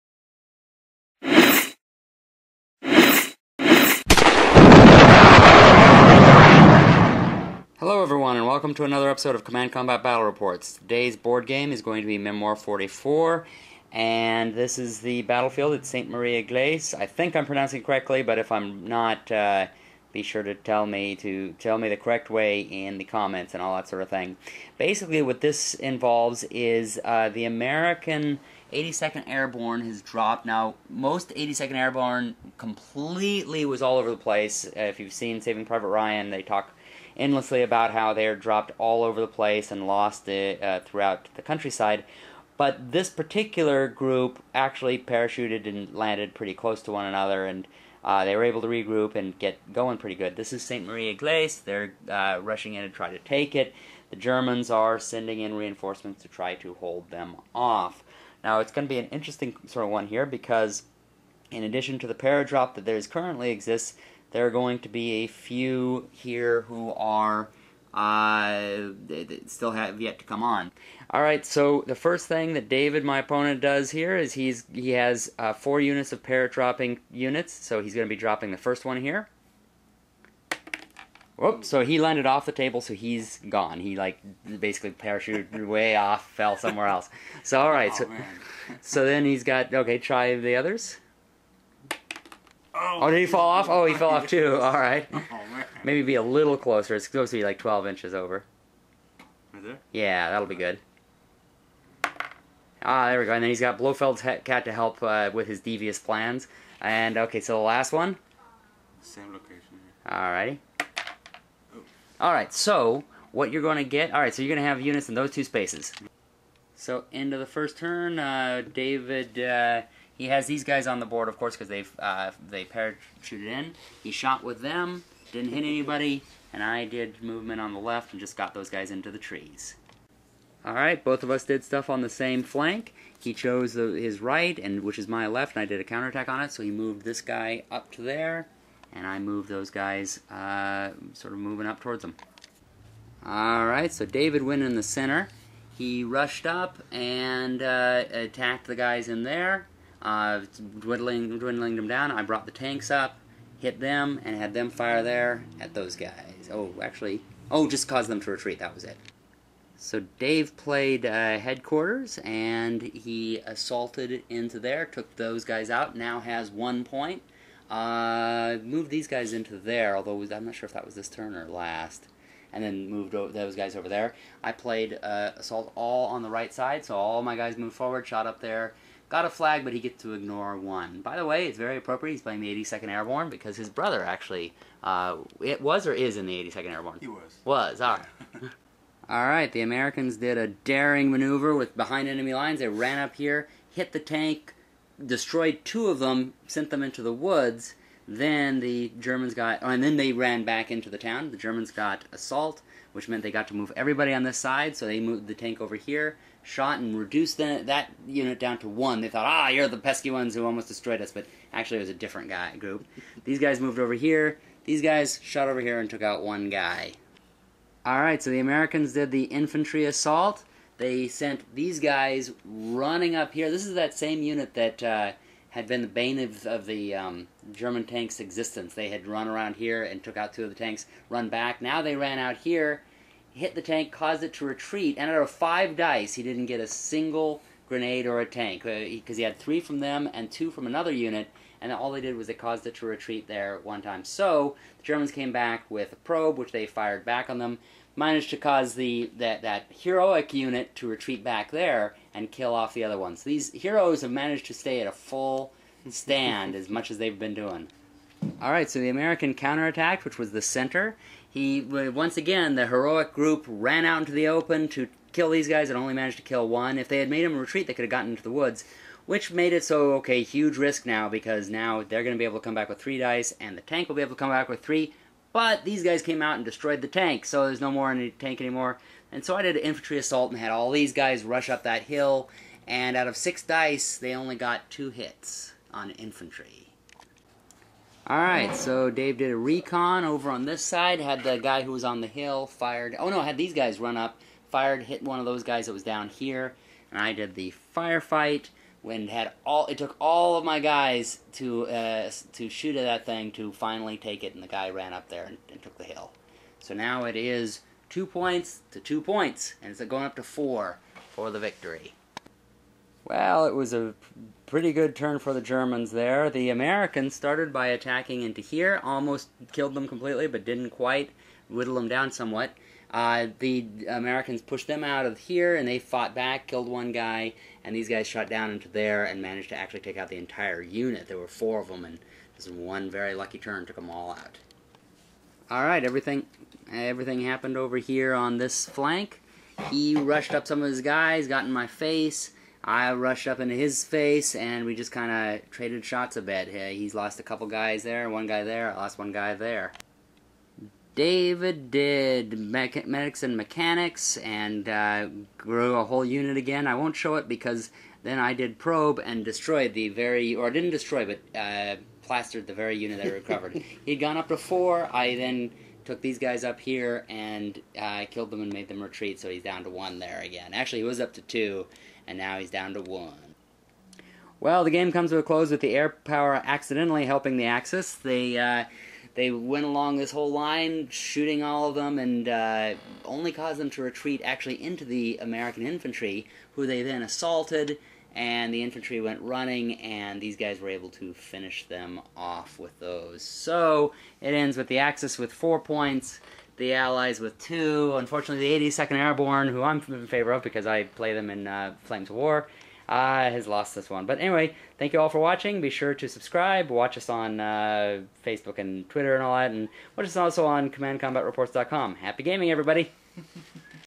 Memoir 44 - St. Mere Eglise
In this game of Memoir 44 we play the Battle of St. Mere Eglise.  In this fight, the 82nd Airborne paradrops deep into German territory.